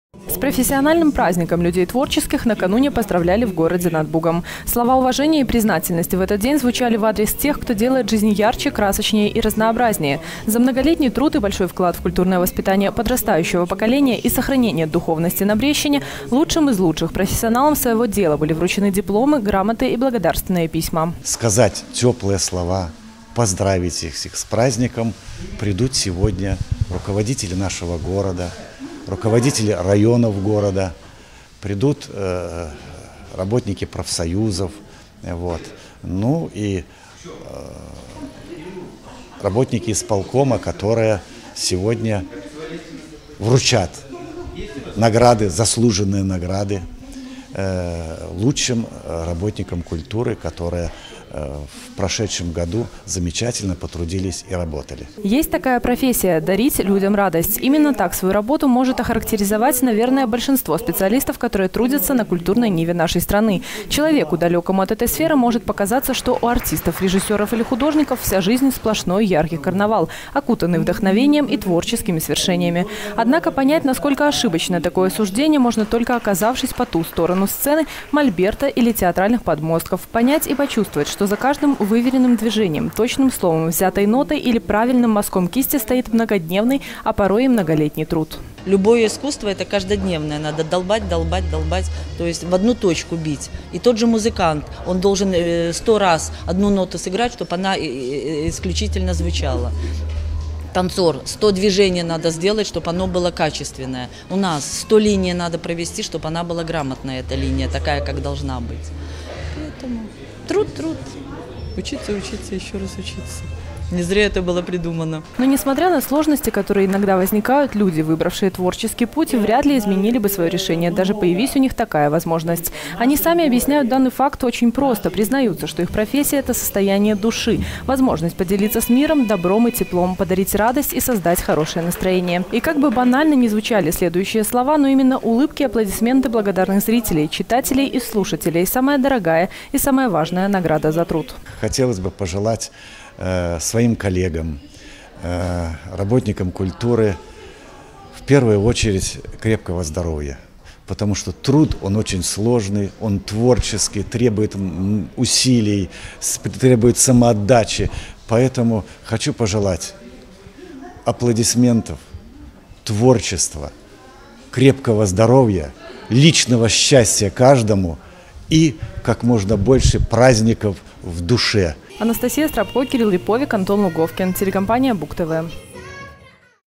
2014-10-10 г. Брест. Городской праздник-чествование работников культуры. Телекомпания Буг-ТВ.
Во второе воскресенье октября в Беларуси свой профессиональный праздник отмечают работники культуры. Торжественные мероприятия, приуроченные к праздничной дате, начались ещё накануне, со слов поздравлений, пожеланий и благодарности за многолетний кропотливый труд. Брестчина заслужено считается краем людей талантливых, творчески одарённых, но для успеха этого зачастую очень мало. И помощь в развитии потенциала детей и молодёжи – это только десятая часть той, которую выполняют те, кого принято называть работниками культуры. Наш корреспондент Анастасия Страпко побывала на городском празднике, и узнала почему никто из виновников торжества, как бы сложно ни было, не изменил бы выбранному творческому пути.